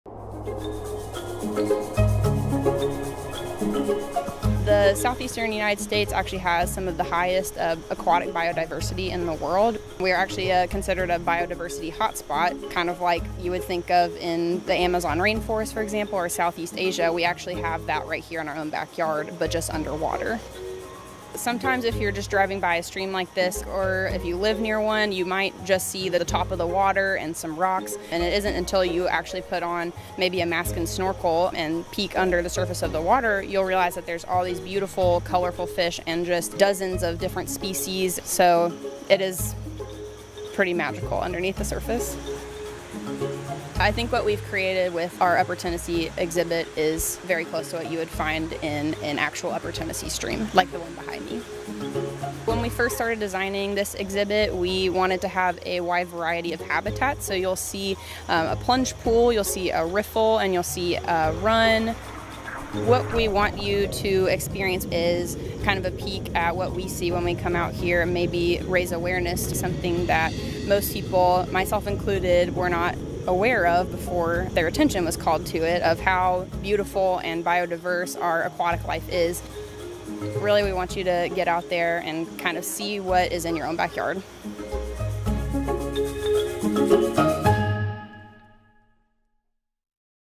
Why We Designed Our Newest Showcase Exhibit to Recreate a Southern Appalachian Waterway
The waterways of Southern Appalachia are brimming with an incredible diversity of aquatic species. This natural abundance has earned the region the distinction among scientists as a hot spot of biodiversity likened to an “underwater rainforest.” What better place to focus our efforts for the centerpiece of our new Ridges to Rivers gallery, a sprawling recreation of a life-filled mountain stream?